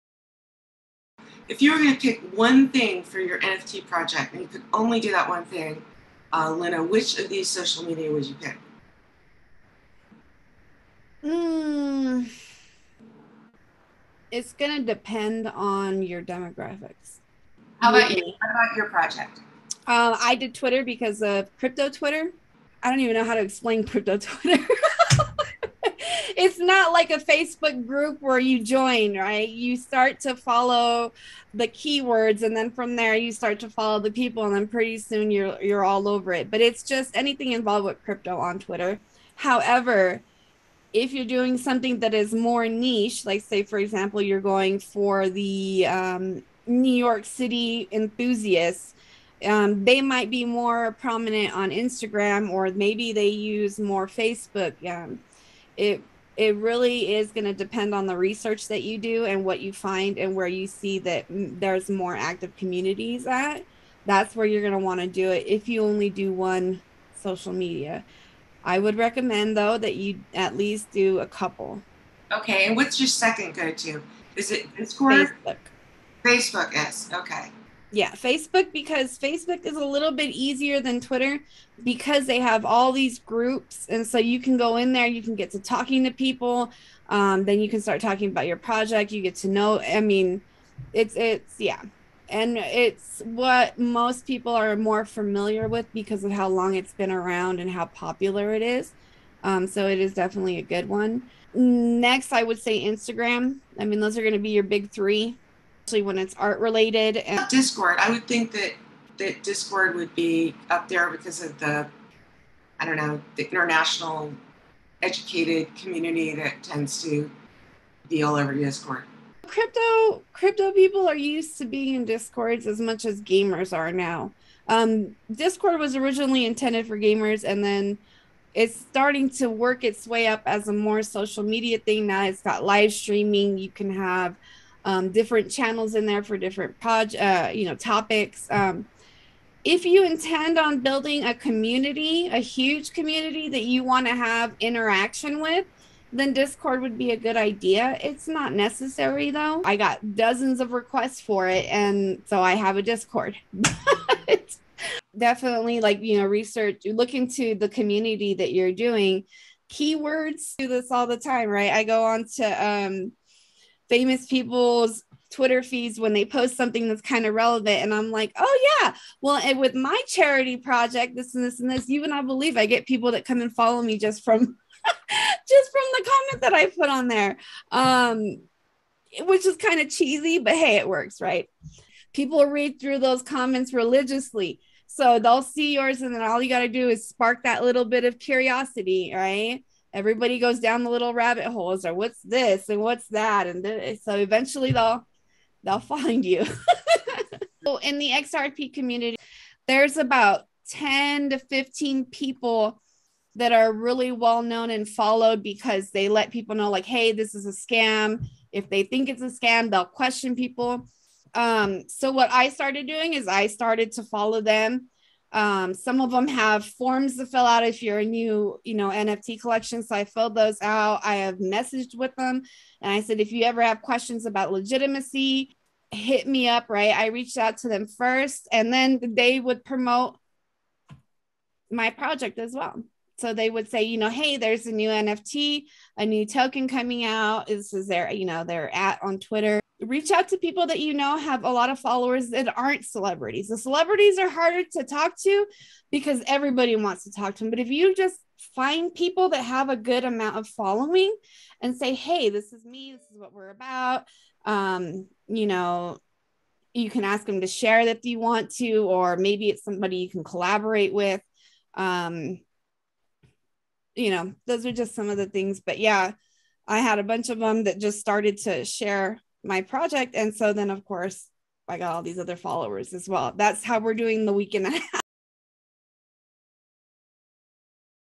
NFT promotions 5: Social Media
Discover which social media channel to use and how to tap into a community that may be interested in purchasing your NFT. Instructor built 5300 followers from scratch in the first two weeks of social promotions. There is a method!